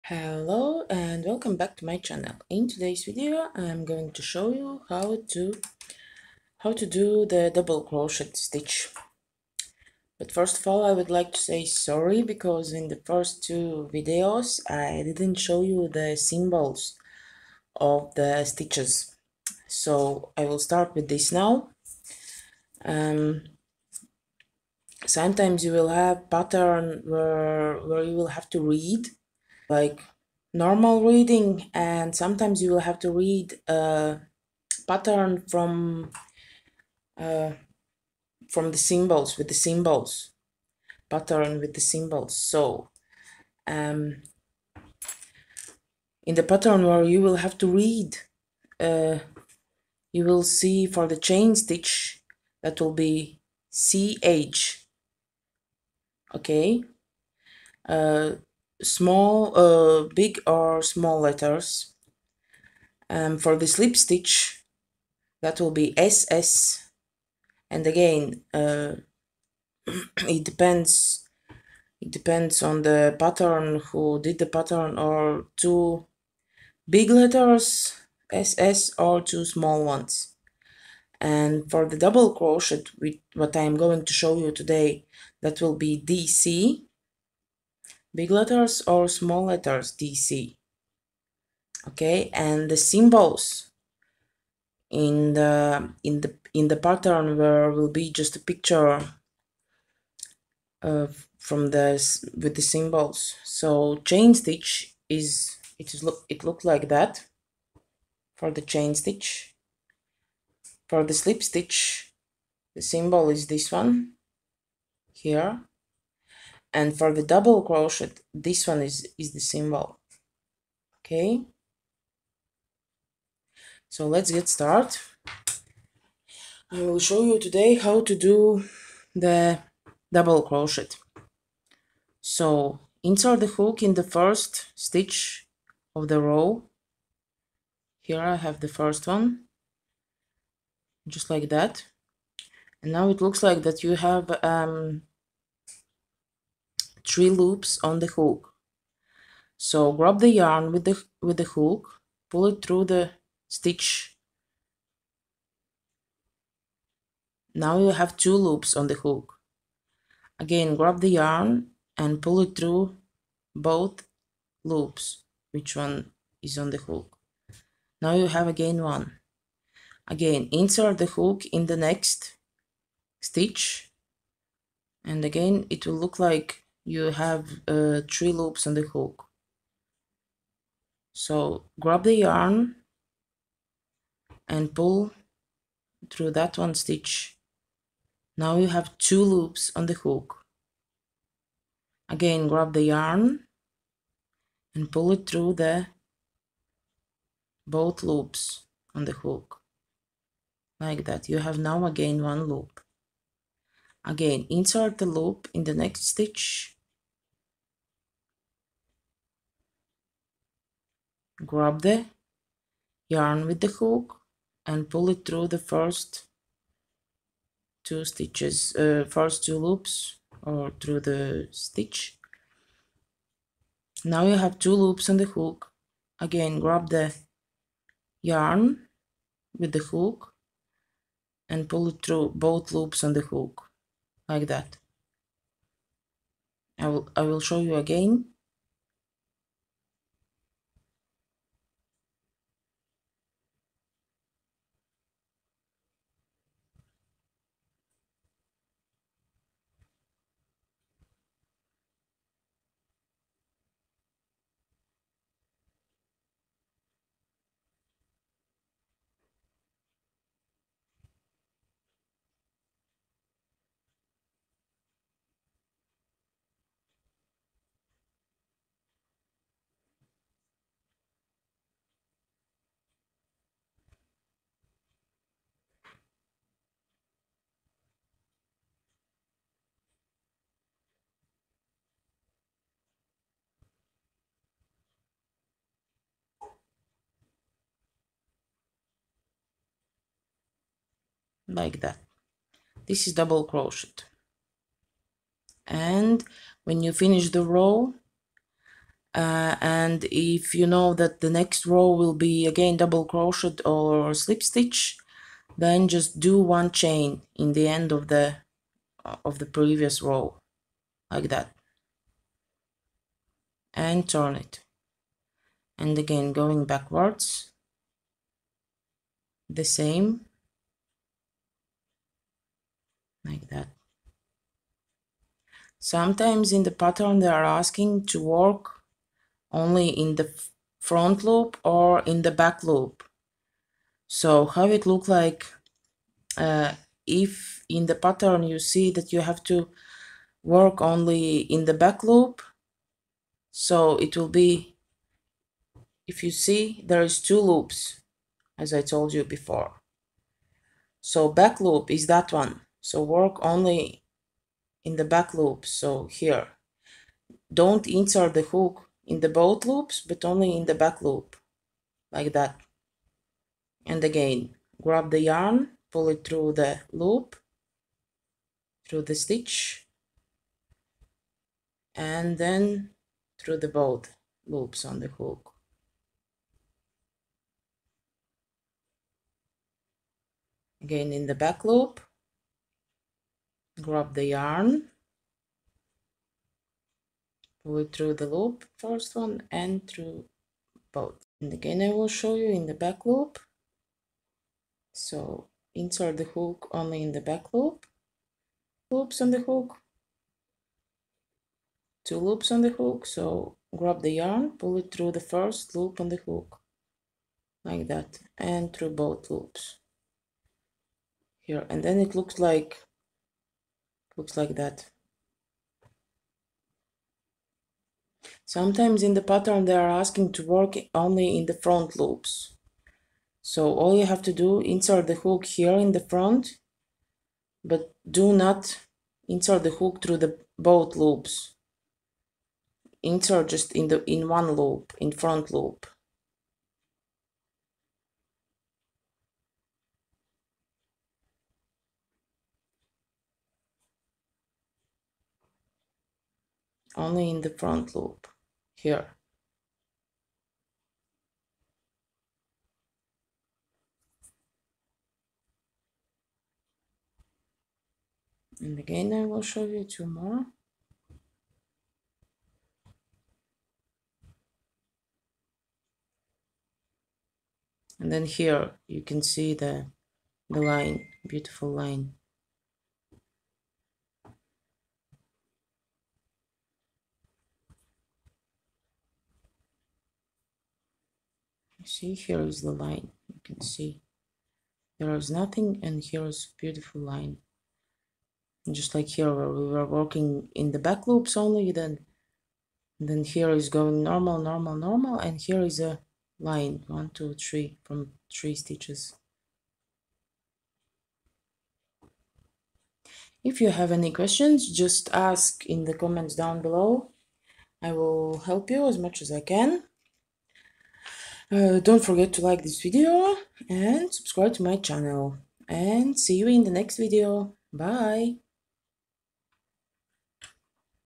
How to crochet for absolute beginners / part 3 / Double crochet; back loops; front loops
In this video you can watch and learn how to do double crochet and how to work only in a back or front loops!